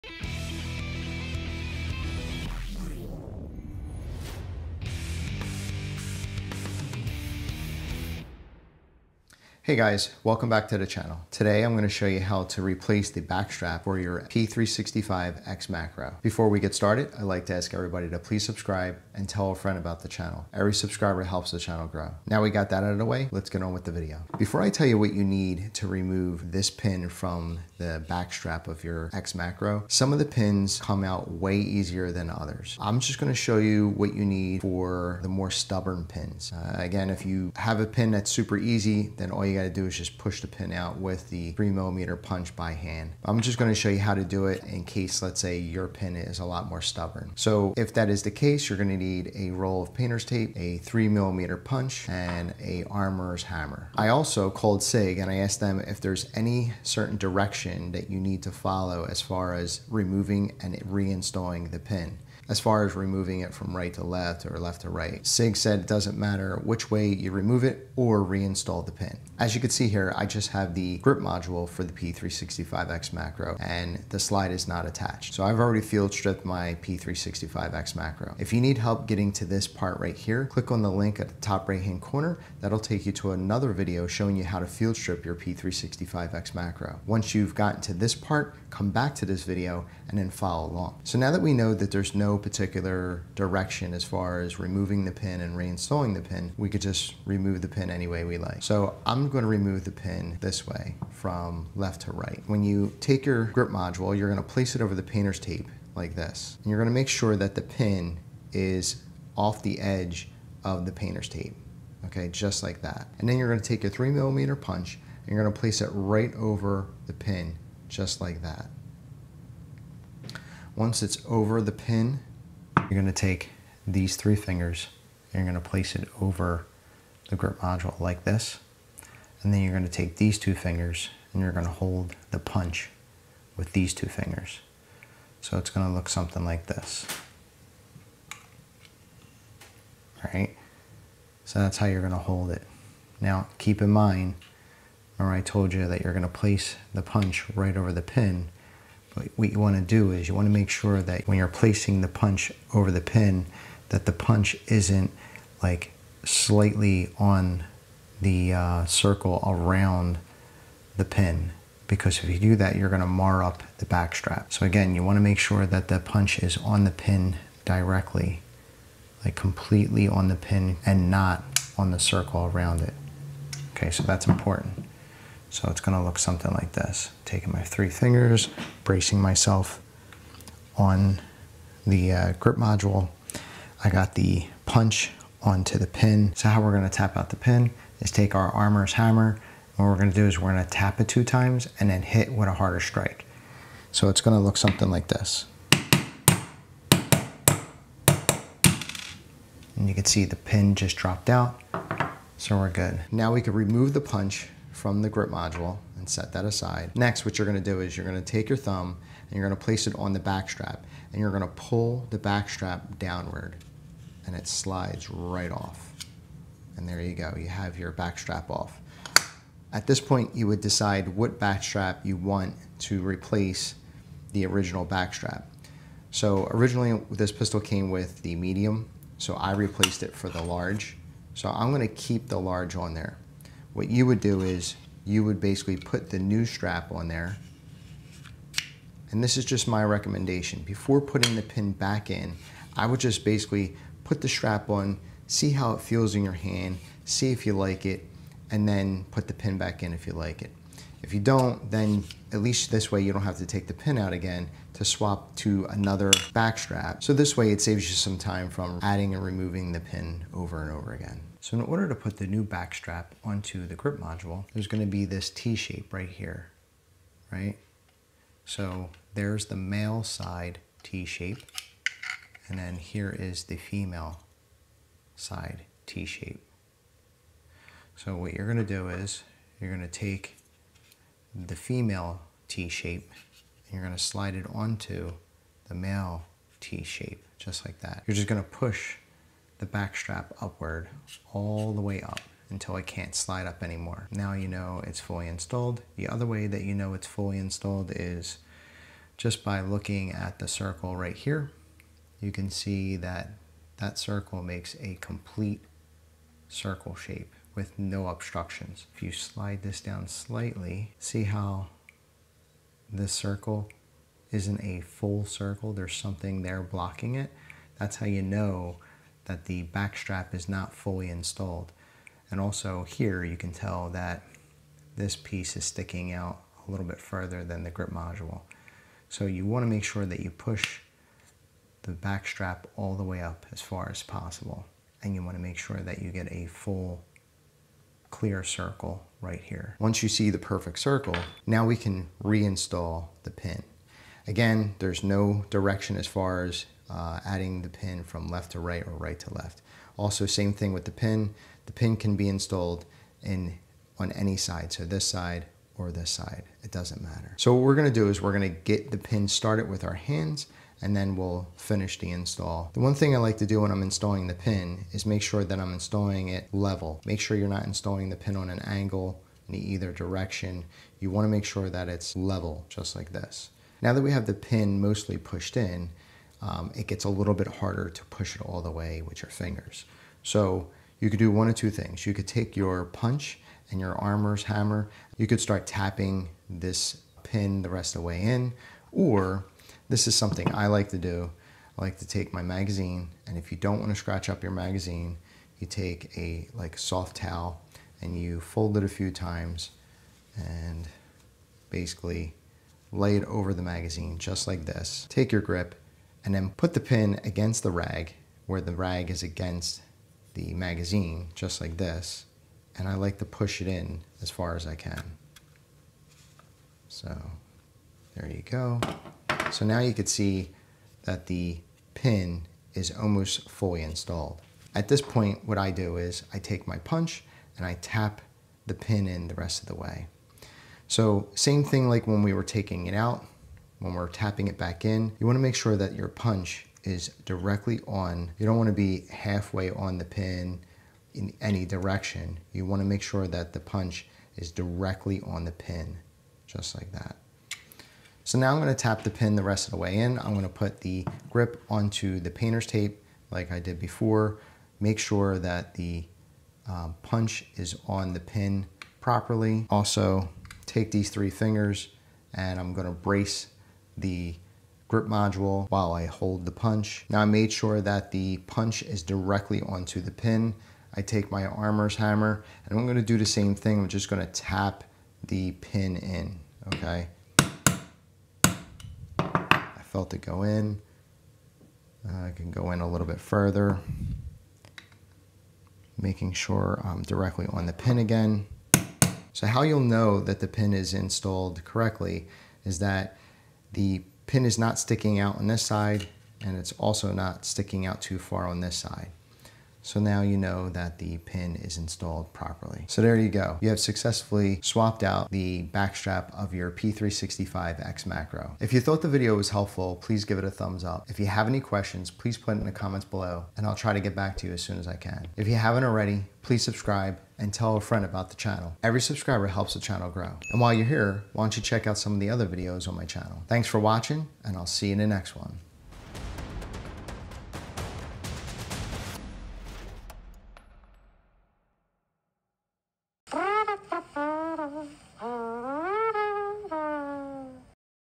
How to Change SIG Sauer P365 XMacro Backstrap
Harry from UBR Tech shows you how easy it is to change the SIG Sauer P365 XMacro's backstrap.

This video is to show you how to assemble and disassemble for purposes of repair or maintenance only.

Tool set: